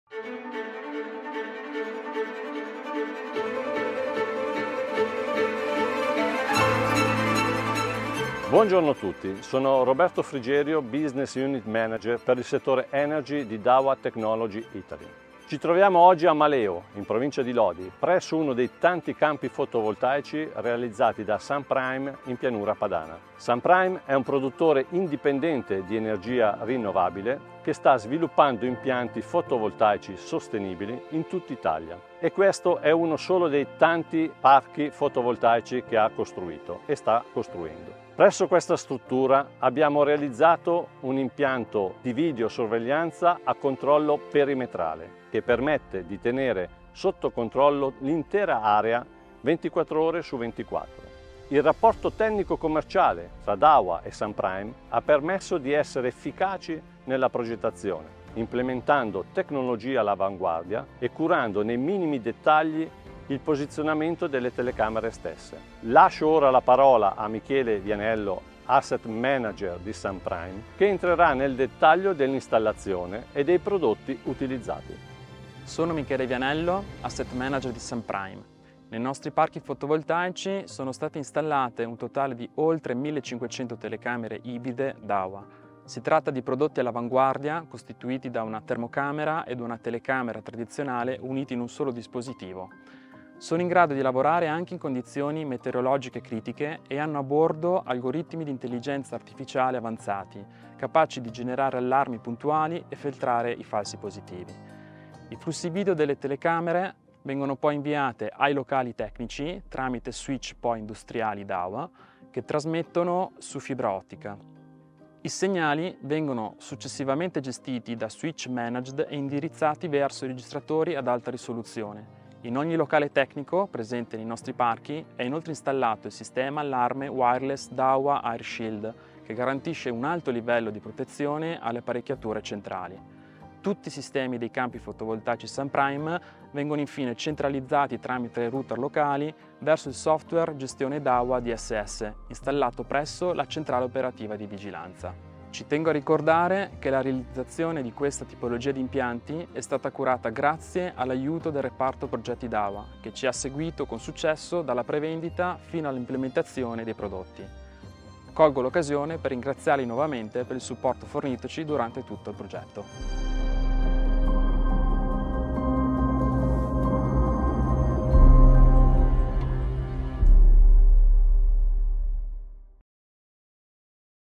Success Case — SUNPRIME Solar Plants Thermal Perimeter Protection Project in Italy
The SUNPRIME Solar Plants Thermal Perimeter Protection Project have constructed over 1500 thermal camera, combined with AI algorithms, could generating precise alerts for intrusion. At the same time, the video steam from the cameras are sent to high-resolution recorders via Dahua industrial switches. In each technical room in solar park, is also installed the wireless alarm system Dahua Airshield. All the systems of Sunprime’s solar parks are finally centralized through local routers to the Dahua DSS management software installed at the surveillance operations center.